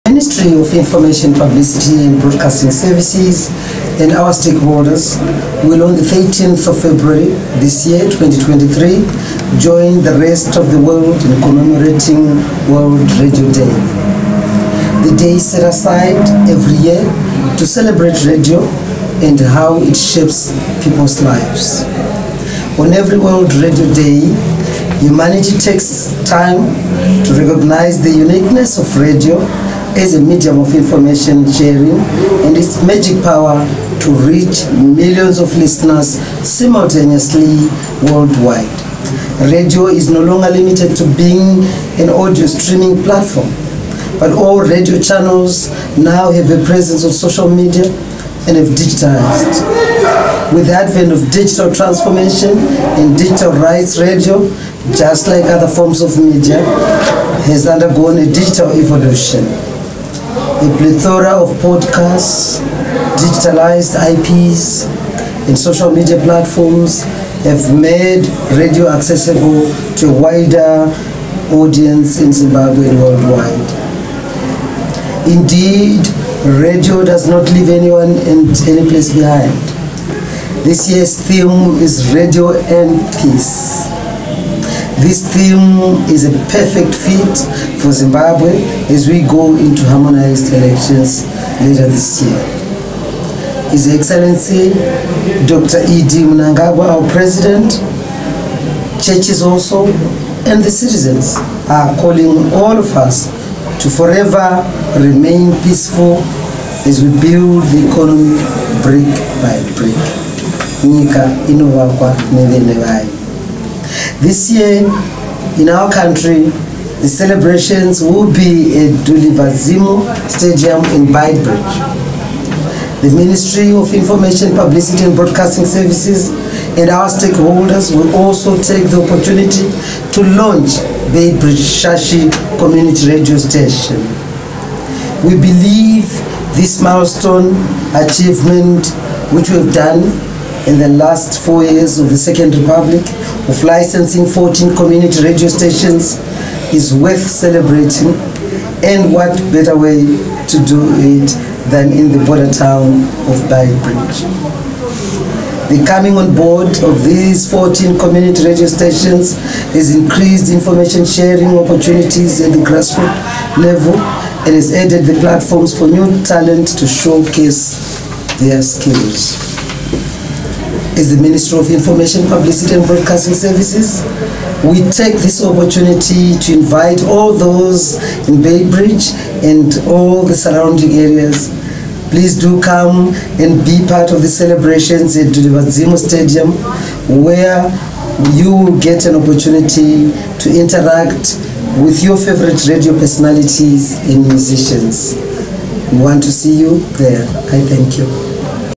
World Radio Day celebrated
The coming on board of 14 community radio stations across the country has increased information sharing opportunities at the grassroots level, while adding platforms for new talent to showcase their skills.

In Manicaland, Chimanimani FM, Nyangani FM, Vemuganga FM and Ndau FM community radio stations were licenced over the past four years and they have been broadcasting since last year